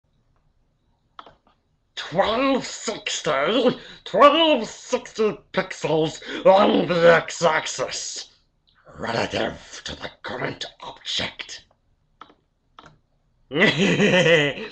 This is why you don't program for 24 hours straight!
You start to go a little crazy!
Filmed while programming Accorid II: Liberation of Siraned